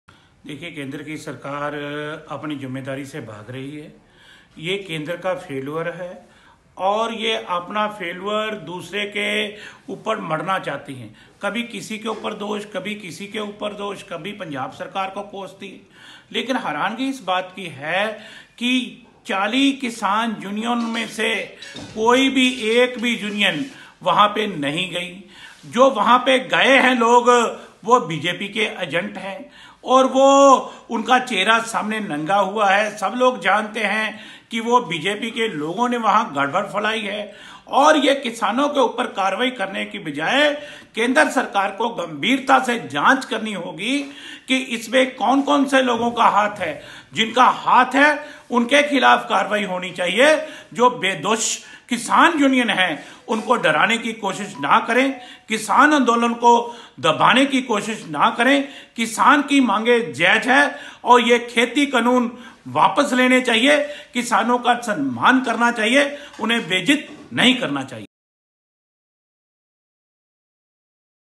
ਕਿਸਾਨ ਅੰਦੋਲਨ ਨੂੰ ਖ਼ਰਾਬ ਕਰਨ ਦੀ ਭਾਜਪਾ ਕਰ ਰਹੀਂ ਕੋਸ਼ਿਸ਼
ਅੰਮ੍ਰਿਤਸਰ: ਵਿਧਾਇਕ ਡਾ. ਰਾਜ ਕੁਮਾਰ ਵੇਰਕਾ ਨੇ ਕਿਹਾ ਕਿ ਕੇਂਦਰ ਦੀ ਮੋਦੀ ਸਰਕਾਰ ਆਪਣੀ ਜਿੰਮੇਵਾਰੀ ਤੋਂ ਭੱਜ ਰਹੀ ਹੈ, ਇਹ ਕੇਦਰ ਦਾ ਫੈਲ ਵਰਕ ਹੈ। ਉਨ੍ਹਾਂ ਕਿਹਾ ਕਿ ਸਰਕਾਰ ਆਪਣਾ ਦੋਸ਼ ਦੂਜੇ 'ਤੇ ਮੜ੍ਹ ਰਹੀ ਹੈ ਕਦੇ ਕਾਂਗਰਸ ਪਾਰਟੀ 'ਤੇ ਦੋਸ਼ ਲਗਾ ਰਹੀ। ਉਨ੍ਹਾਂ ਕਿਹਾ ਕਿ ਹੈਰਾਨੀ ਇਸ ਗੱਲ ਦੀ ਹੈ ਕਿ 40 ਕਿਸਾਨ ਜਥੇਬੰਦੀਆਂ ਵਿੱਚੋਂ ਕੋਈ ਵੀ ਲਾਲ ਕਿਲ੍ਹੇ ਨਹੀਂ ਗਈ, ਜਿਹੜੇ ਉੱਥੇ ਗਏ ਸਨ ਉਹ ਬੀਜੇਪੀ ਦੇ ਲੋਕ ਸਨ। ਸਰਕਾਰ ਨੂੰ ਕਿਸਾਨਾਂ 'ਤੇ ਕਾਰਵਾਈ ਕਰਨ ਦੀ ਬਜਾਏ ਇਸ ਦੀ ਕੇਂਦਰ ਸਰਕਾਰ ਨੂੰ ਗੰਭੀਰਤਾ ਨਾਲ ਸਹੀ ਤਰੀਕੇ ਨਾਲ ਜਾਂਚ ਕਾਰਵਾਈ ਜਾਣੀ ਚਾਹੀਦੀ ਹੈ।